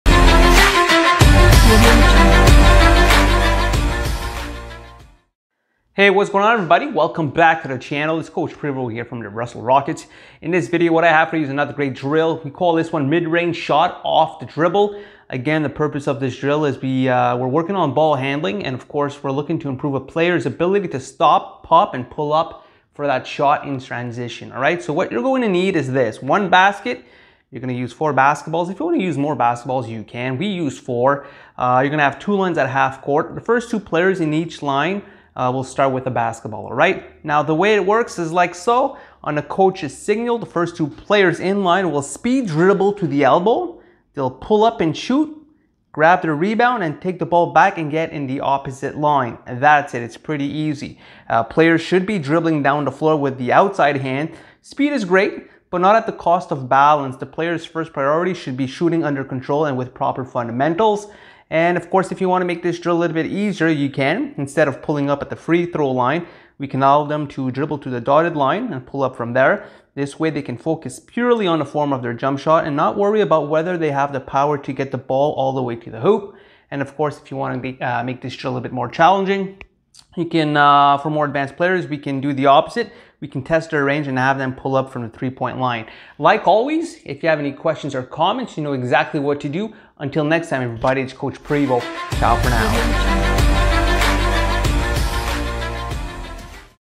MID RANGE SHOT OFF DRIBBLE DRILL
1)  On coaches signal thefirst 2 players in line will speed dribble to the elbow.

2)  They will pull up and shoot grab their rebounds then take the ball back and get in the other line.